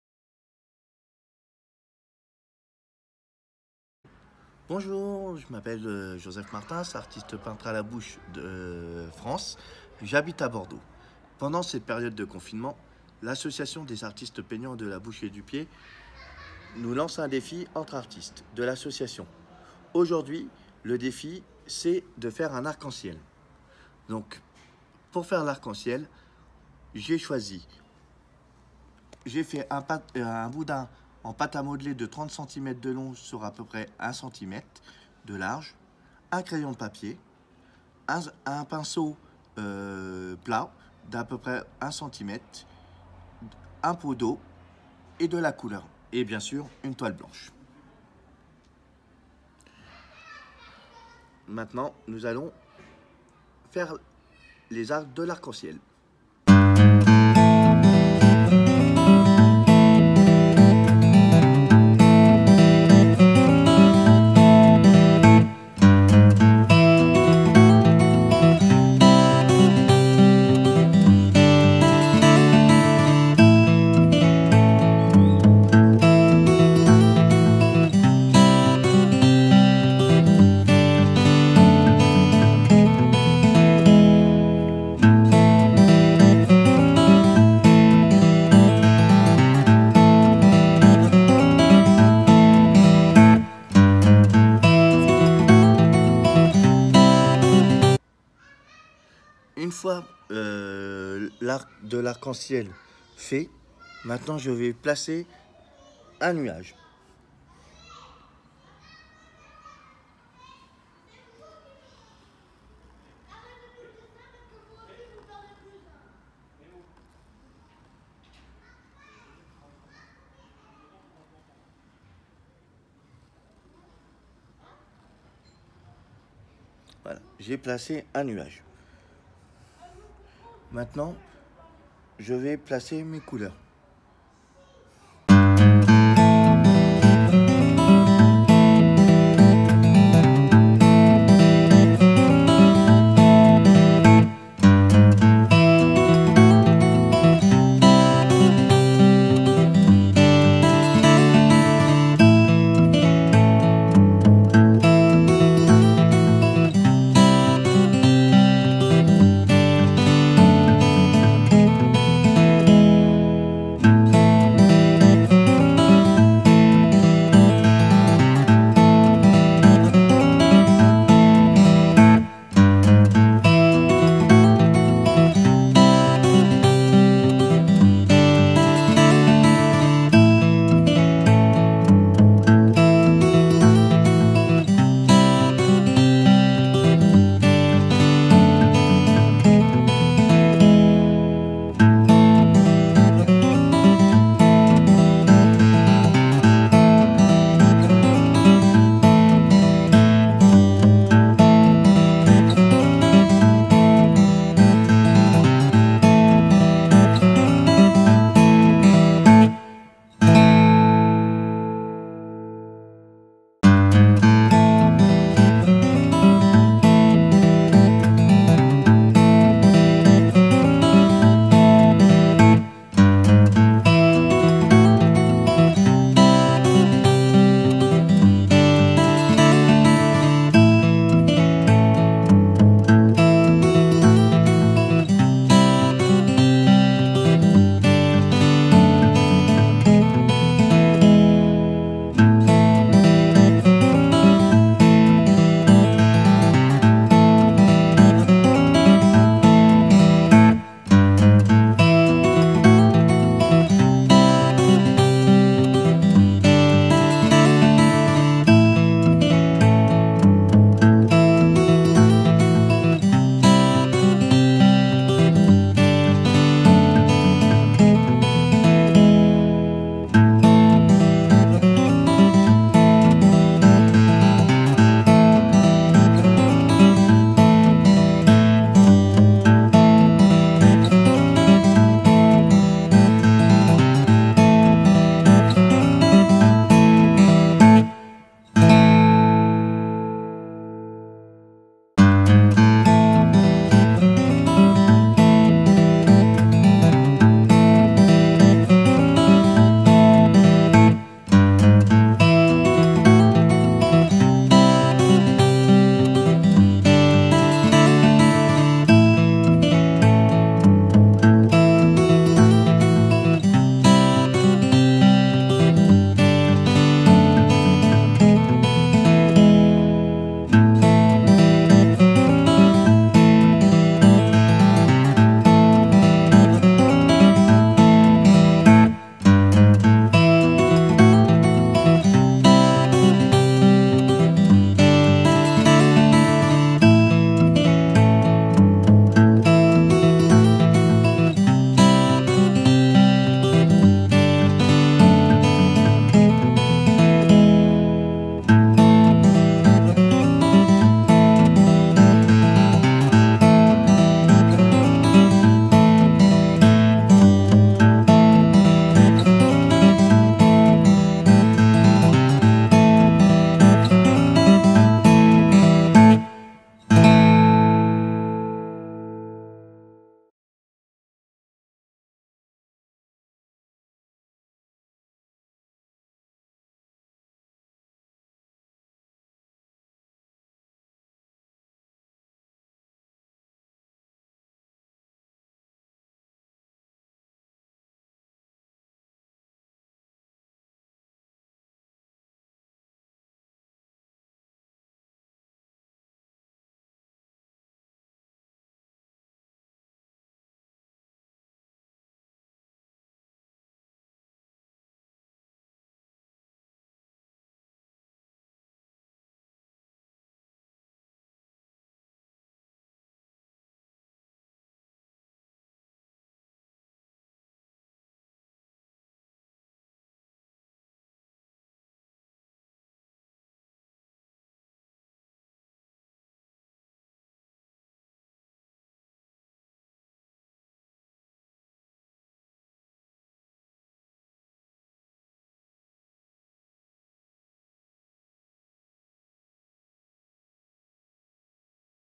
TUTO RAINBOW by Joseph Martins
TUTO RAINBOW

APPRENEZ A REALISER SIMPLEMENT UN ARC-EN-CIEL , 7 MN pour réaliser votre arc-en-ciel pour dire que TOUT IRA BIEN !

Joseph artiste peintre à la bouche , expose et travaille sur Bordeaux , il aime partager avec vous sa passion , la peinture .